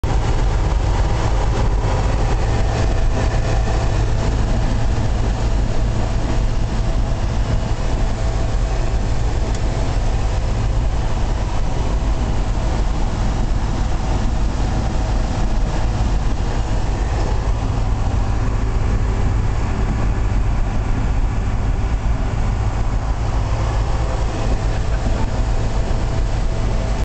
the drive to harper
driving 200 miles in a 1965 land rover series 2a to harper university on the motorway. cruising at 60 reaching 70 when overtaking